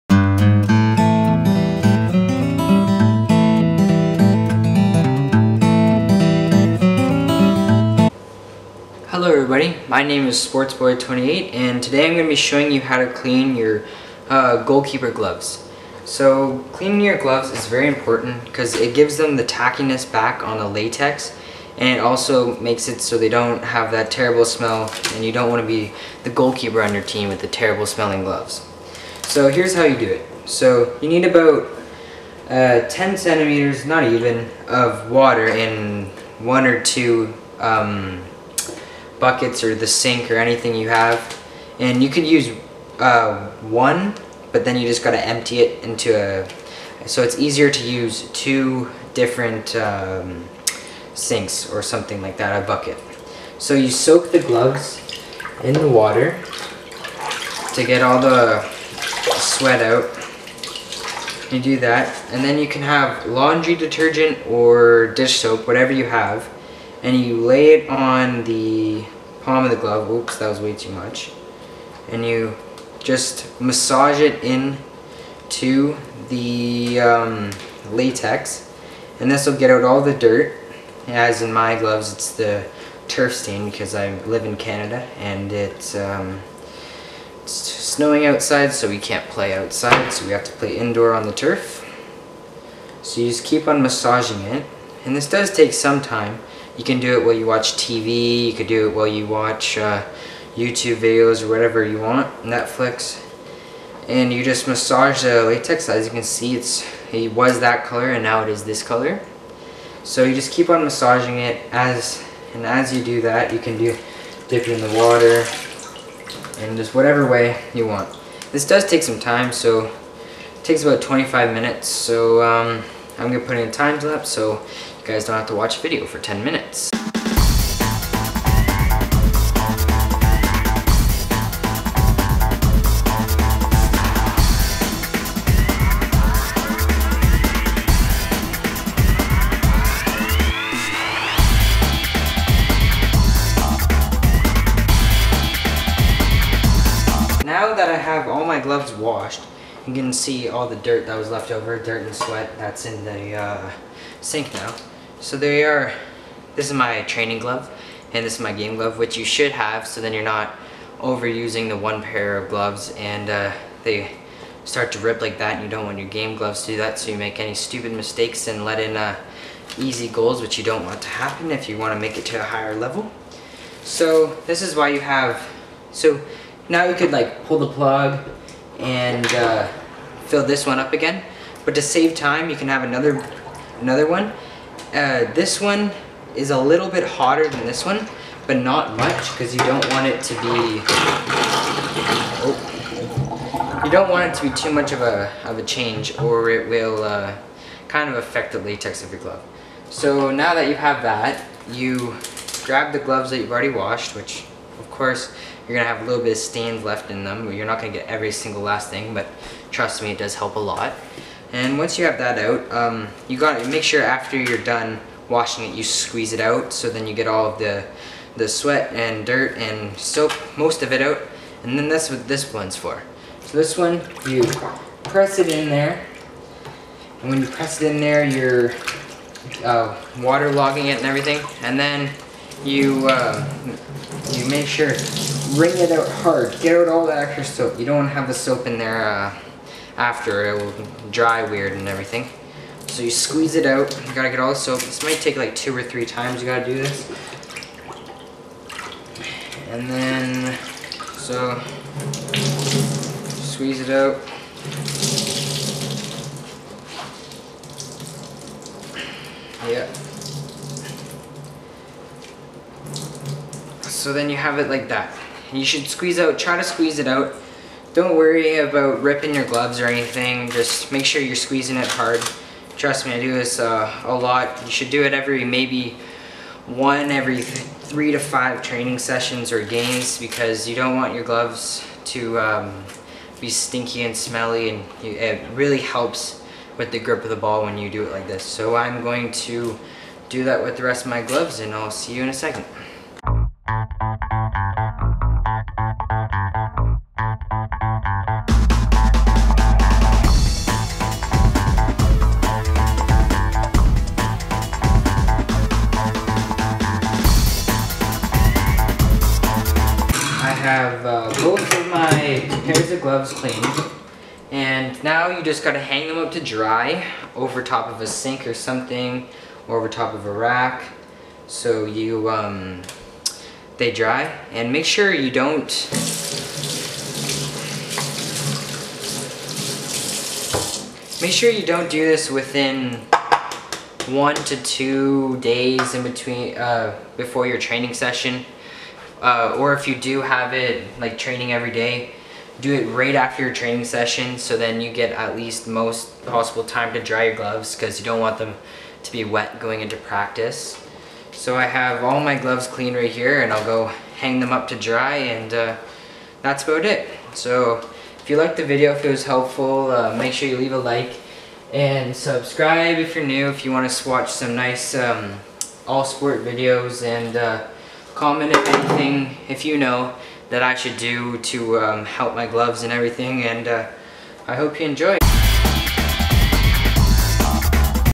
How to clean your GOALKEEPER gloves!!!
Here are some tips to clean your goalkeeper gloves
- Wash very 3-5 training sessions
- Hang dry not blow dry
- do not use burning hot water use room temperature
I hope you enjoyed and this video helped you.